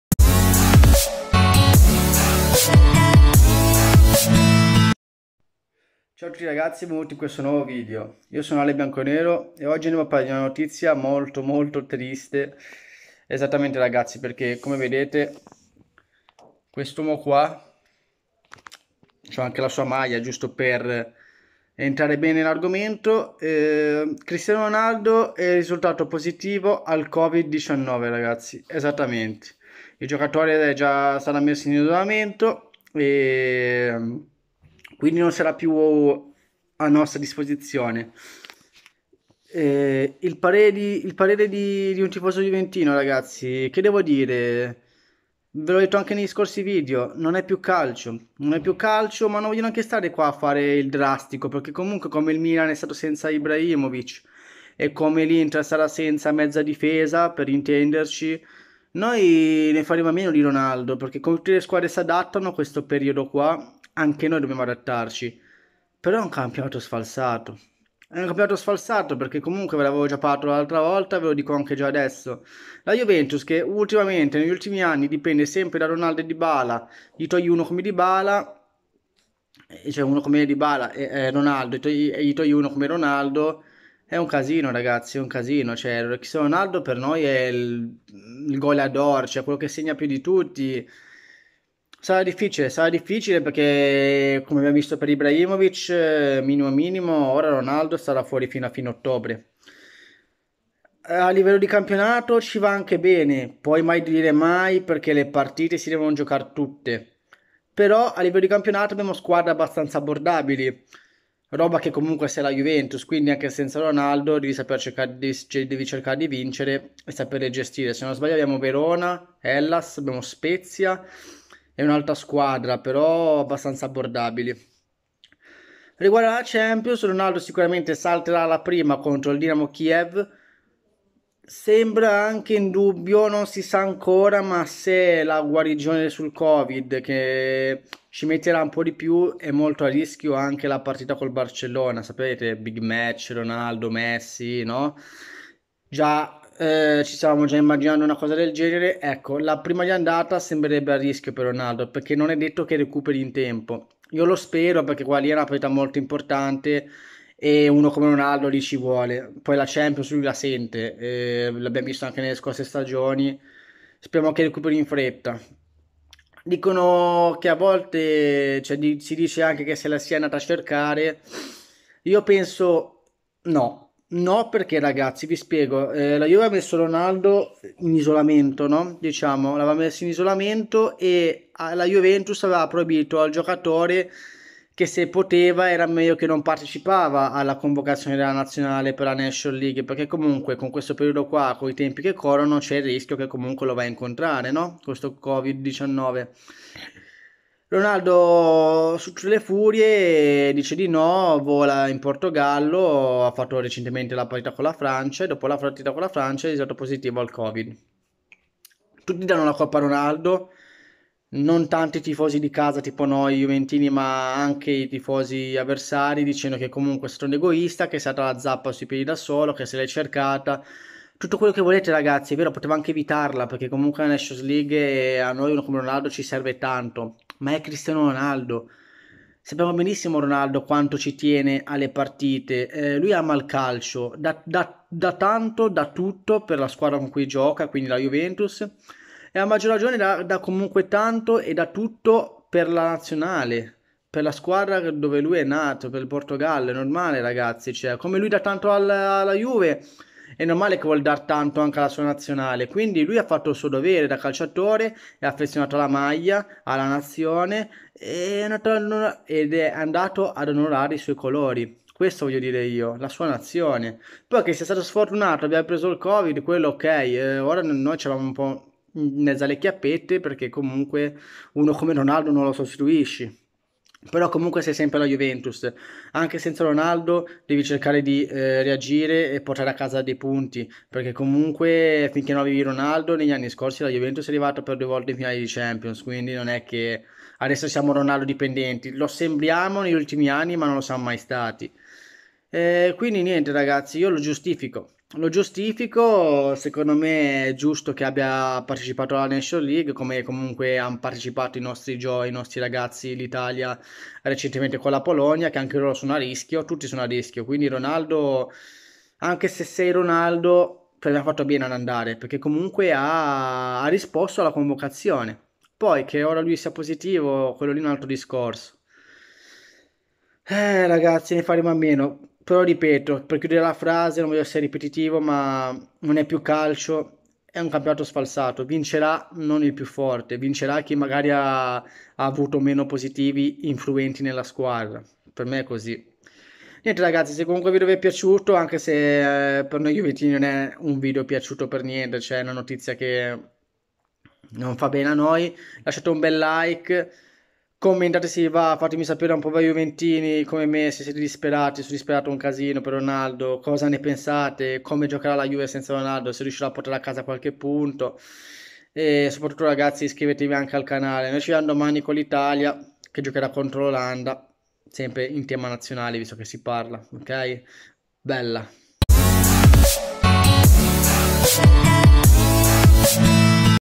RONALDO POSITIVO AL COVID-19! LA REAZIONE DI UN TIFOSO JUVENTINO 😢
Il fuoriclasse portoghese della Juventus è risultato positivo al Covid-19 e salterà la sfida di Nations League in programma mercoledì tra Portogallo e Svezia. Il giocatore è asintomatico ed è stato subito posto in isolamento. Secondo le nuove norme del Governo italiano dovrà attendere dieci giorni e un tampone negativo (e salterà minimo le sfide contro il Crotone e la Dinamo Kiev). CR7 trascorrerà il periodo di isolamento in Portogallo.